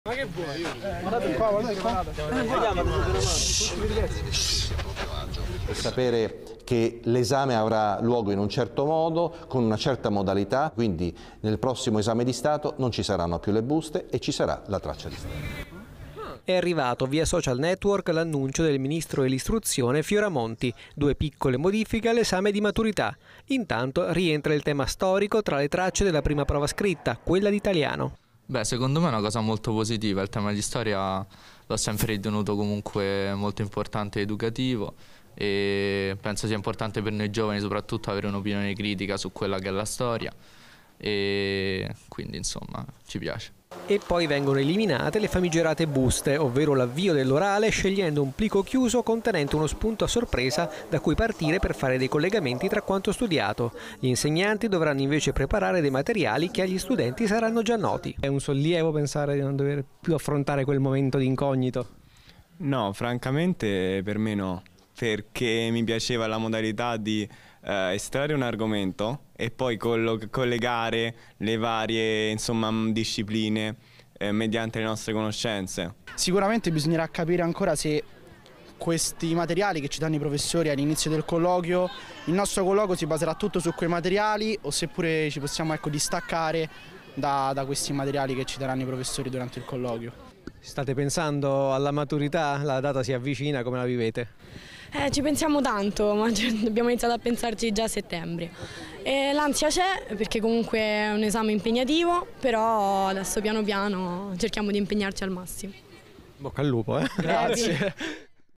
Nuova maturità, ecco come cambia. Il parere degli studenti
Le novità dell'esame di Stato che quest'anno si svolgerà il 17 giugno. Si discute tra gli studenti sui contenuti dell'ennesima riforma. Primi fra tutti, il ritorno del tema di storia e l'addio alle buste con gli argomenti per l'orale. Servizio di Pierluigi Vito.